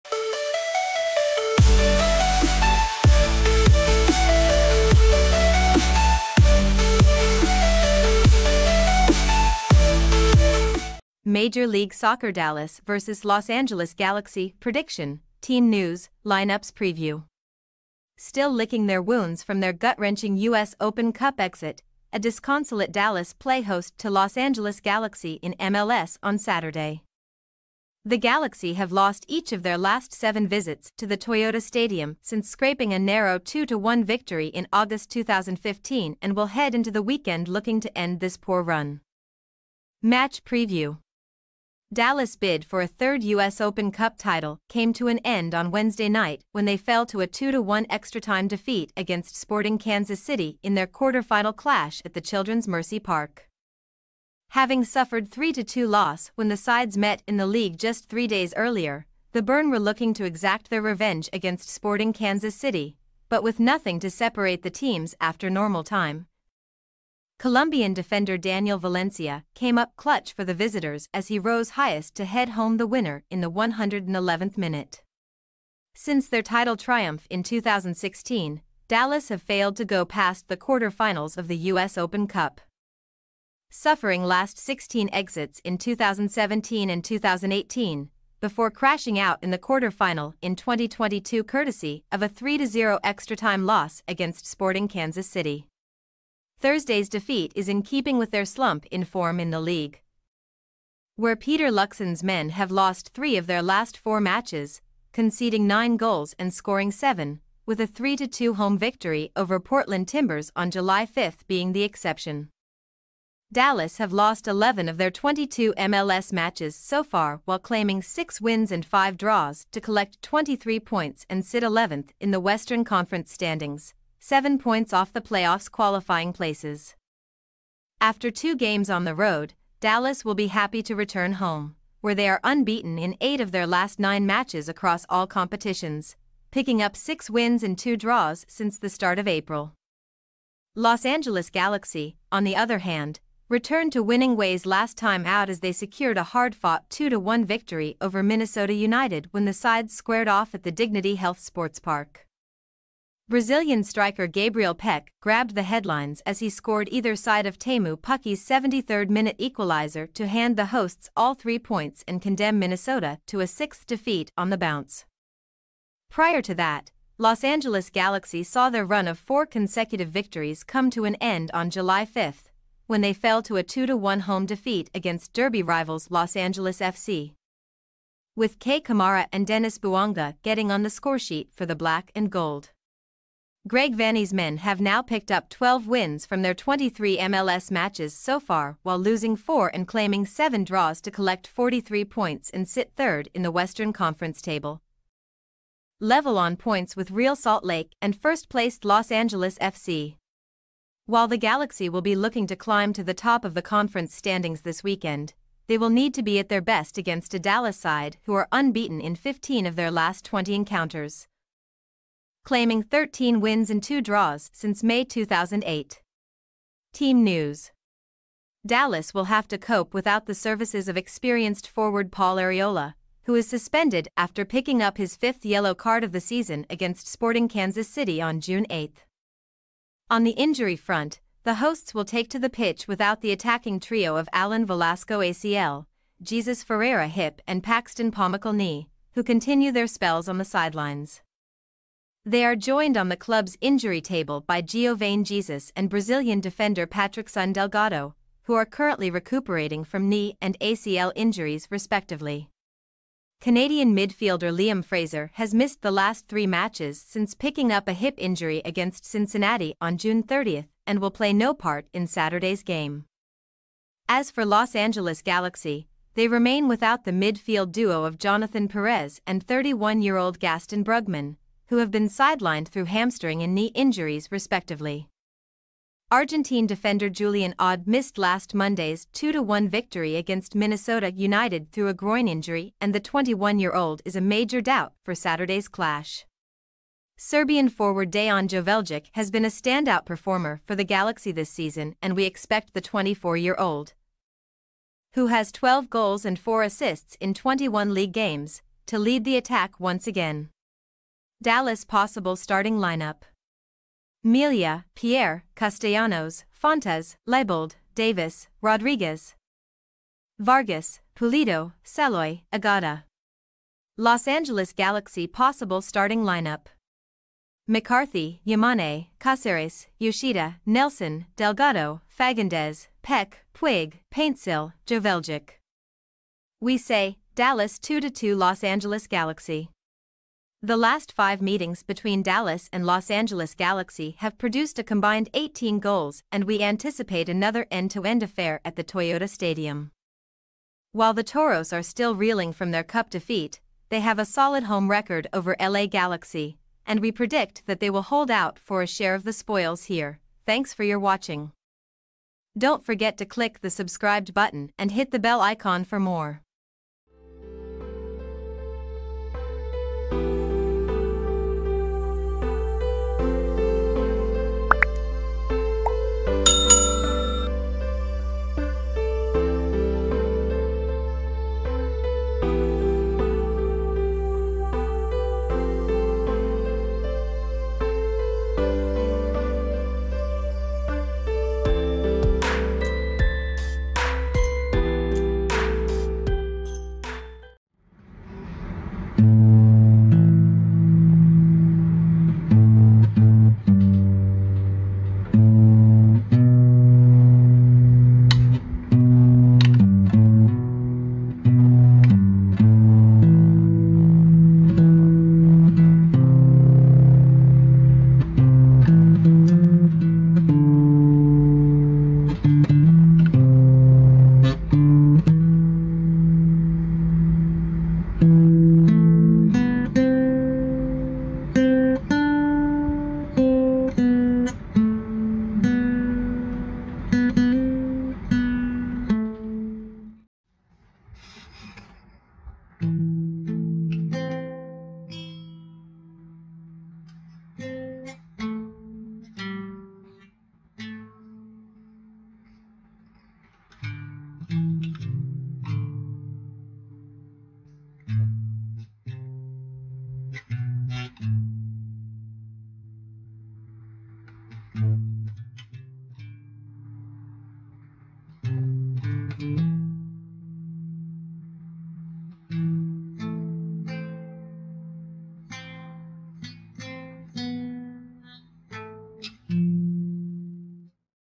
Major League Soccer | Dallas vs. Los Angeles Galaxy - prediction, team news , lineups | Preview
Still licking their wounds from their gut-wrenching US Open Cup exit, a disconsolate Dallas play host to Los Angeles Galaxy in MLS on Saturday.

00:00 Introduction
00:11 Contents